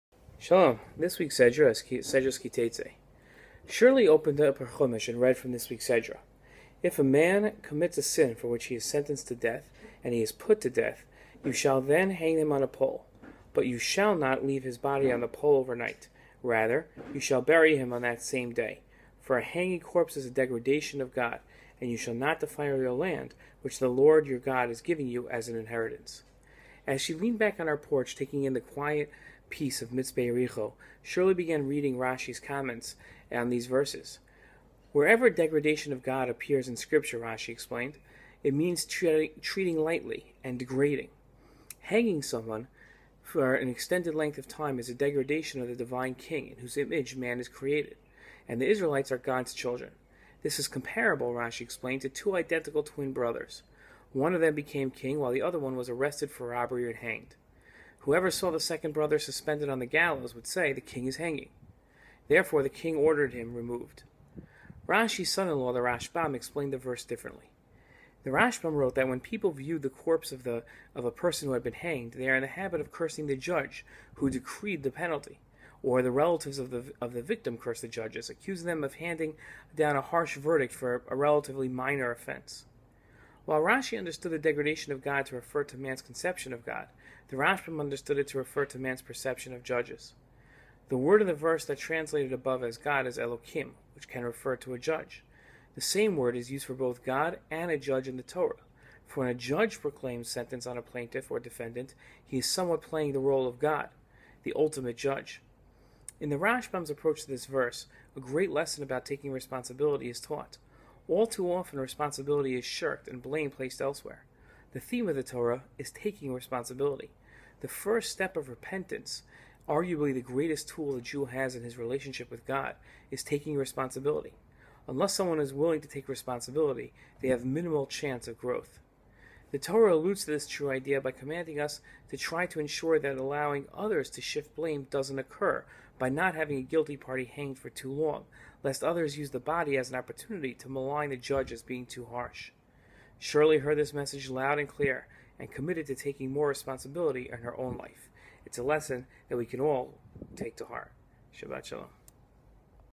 The Sedra Ki Seitzei 5775
The theme of the Torah is taking responsibility. The first step of repentance, arguably the greatest tool of the Jew in their relationship with God, is taking responsibility. Unless someone is willing to take responsibility, they will have minimal chance of growth.